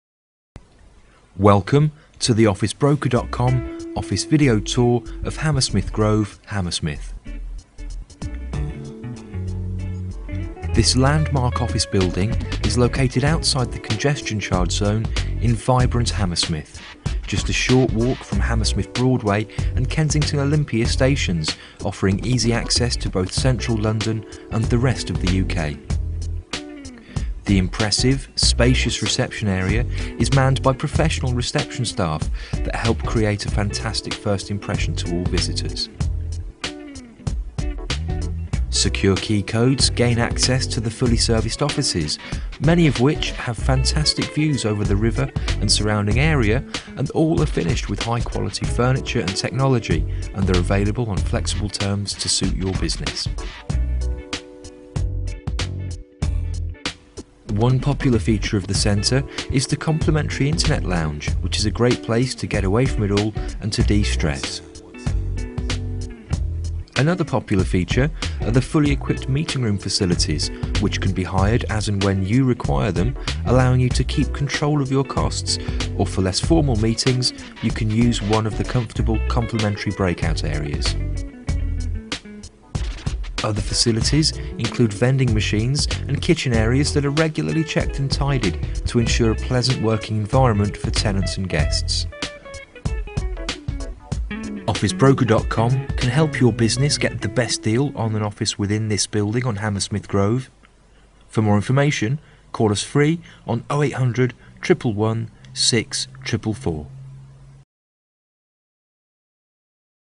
Serviced Office Space - Hammersmith, London W6
Welcome to the officebroker.com serviced office property video tour of Hammersmith Grove, Hammersmith W6.

This Landmark serviced office property is in one of the most popular commerce locations outside the congestion charge zone: vibrant Hammersmith. Surrounded by useful amenities and public transport links inc banks, retail stores and restaurants, and it is also under 1 mls from Hammersmith Broadway and Kensington Olympia stations offering flexible access to both central London and the rest of the UK.
The impressive, open-plan reception area is managed by executive reception staff that help create a fantastic first impression to all visitors to this popular business centre.

Security keycodes gain access to the fully serviced offices, many of which have fantastic views over the river ad surrounding area and all are finished with high quality cat 5 cabling, chairs, desks, telecoms and commercial standard broadband which are managed by the management team of the property for extra convenience. All office suites are available to rent on flexible terms to suit your commercial business needs.

One popular feature of the property is the complimentary internet lounge which is a great place to get away from it all and de-stress whilst taking advantage of the wireless broadband service

Another popular feature are the fully equipped executive meeting room facilities complete with director style chairs which can be rent ed as and when you require them allowing you to keep control of your corporate costs, or for less formal meetings you can use one of the comfortable, complimentary break out areas.

Other facilities inc vending machines and kitchen areas which are regularly checked and tidied to ensure a pleasant environment for tenants and guests.

Officebroker.com can help your business get the best deal on office space within this Hammersmith serviced office property on Hammersmith Grove, Hammersmith.